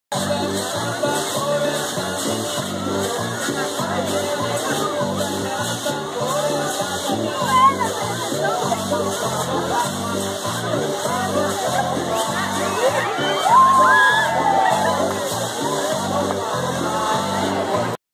El Tagada chimbombin Electrico
Chimbombintv presente en el Tagada en la feria Agostina 2015